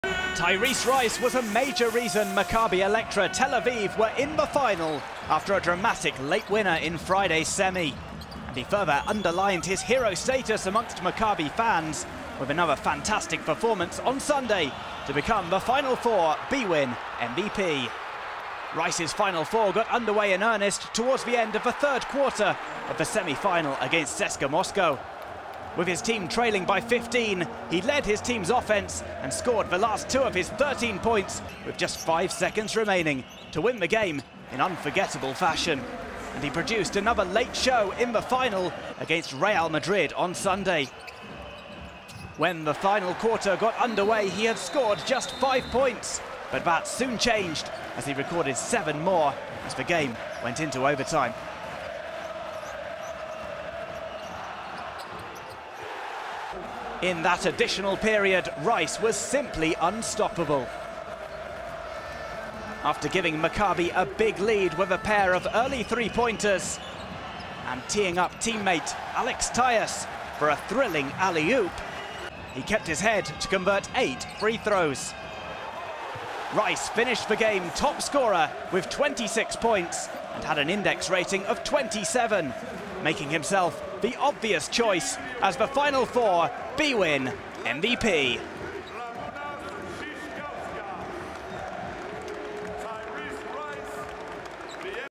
Final Four bwin MVP: Tyrese Rice, Maccabi Electra Tel Aviv
Tyrese Rice was a major reason Maccabi Electra Tel Aviv were in the final after a dramatic late winner in Friday's semi-final, and he further underlined his hero status amongst Maccabi fans with another fantastic performance in the Final on Sunday to become the Final Four bwin MVP.